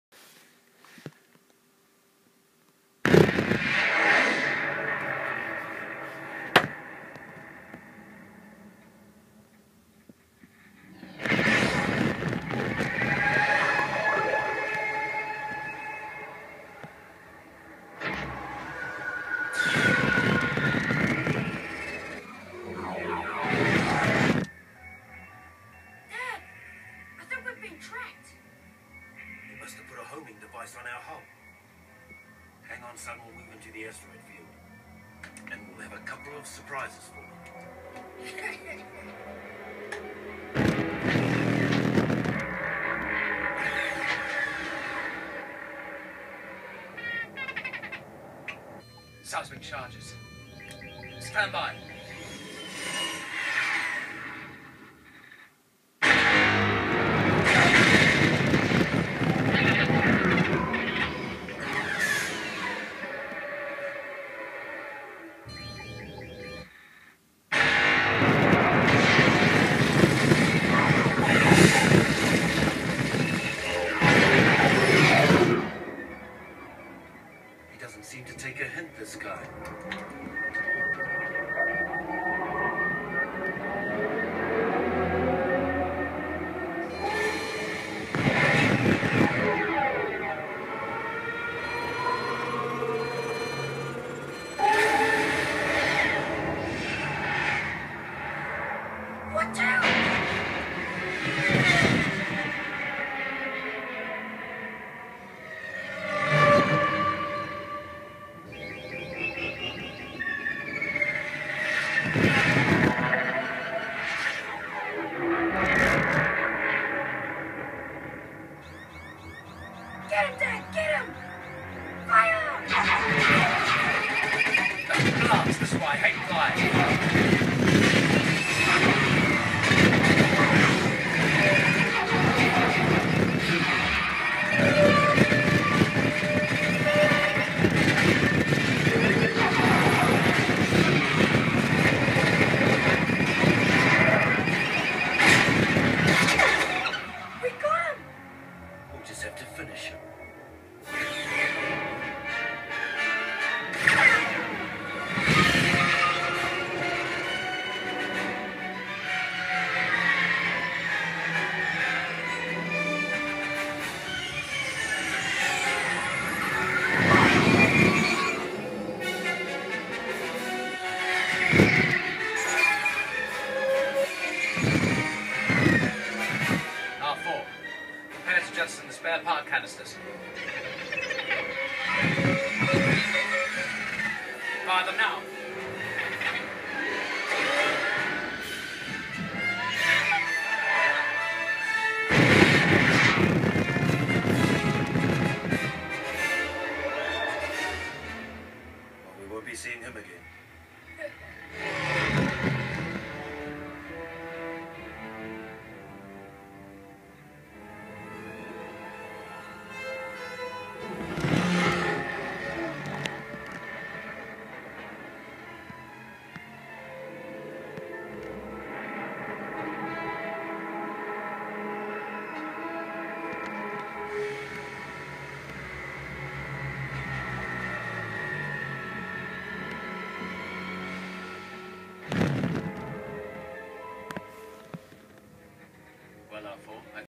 In movie Aura Bass shaker demo Buffalo home theater
The rattling you hear is the ipad vibrating against the seat. The demo works a little better if you keep your eyes on the surrounding environment, like the movie box. I had 2 fluorescent over head lamps on. This just goes to show you how bright the epson home cinema 3010 is.